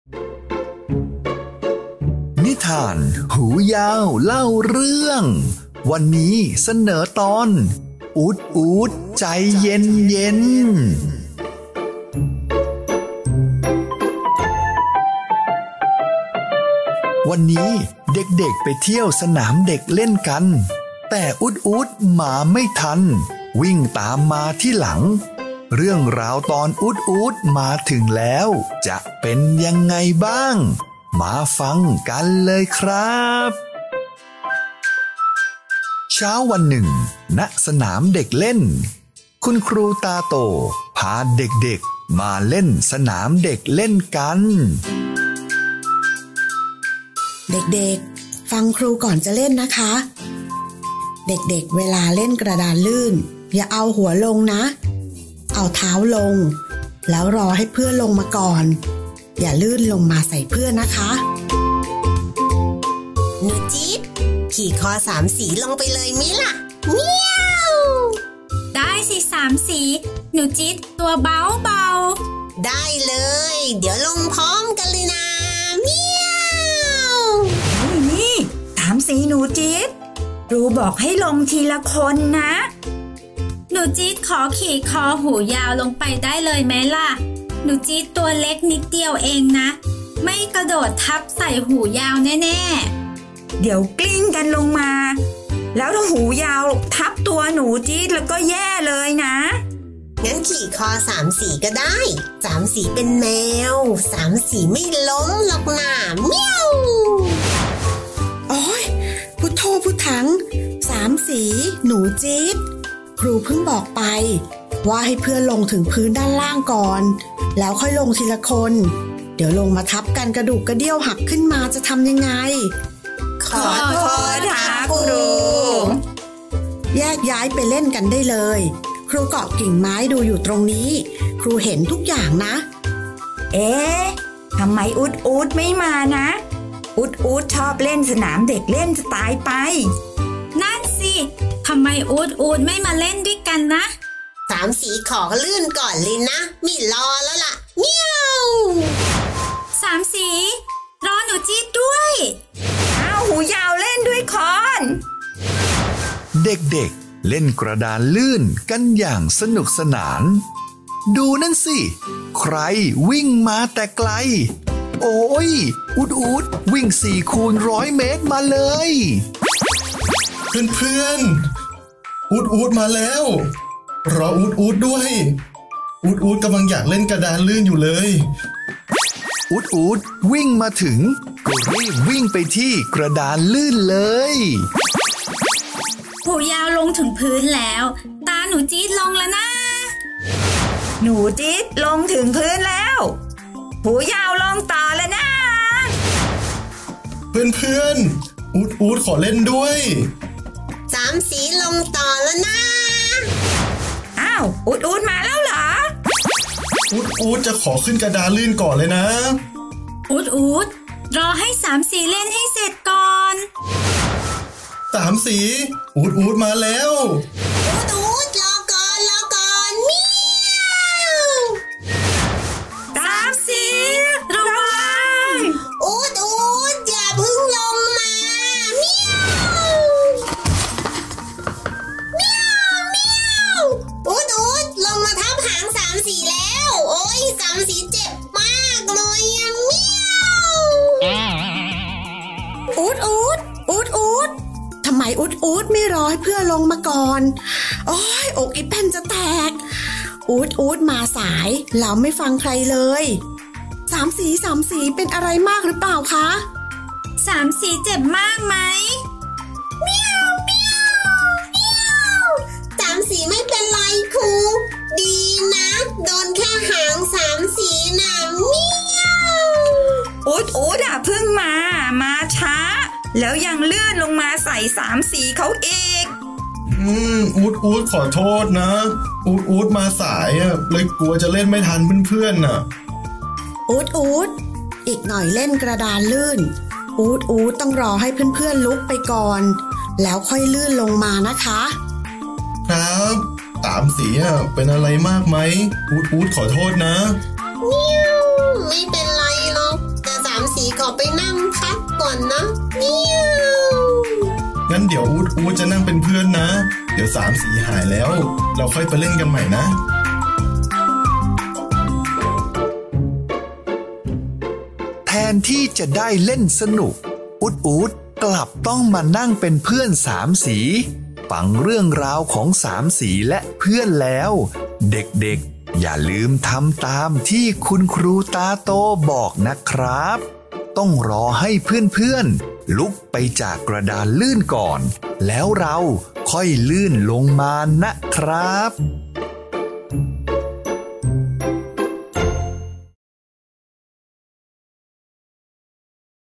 นิทาน อู๊ด ๆ ใจเย็น ๆ | หูยาวเล่าเรื่อง
วันนี้คุณครูพาเด็ก ๆ ออกมาเล่นกระดานลื่นที่สนามเด็กเล่น และบอกกฏกติกาการเล่นร่วมกันให้เด็ก ๆ รู้ คือ ห้ามปีนขึ้นจากด้านข้าง ห้ามเอาหัวลง รอให้เพื่อนลุกก่อน แล้วค่อยเล่นต่อ

แต่อู๊ด ๆ มาสายเลยรีบวิ่งมาเล่นกับเพื่อน ๆ ทั้งที่เพื่อนยังเล่นไม่เสร็จ แบบนี้เพื่อนก็เจ็บตัวแย่เลยน่ะสิ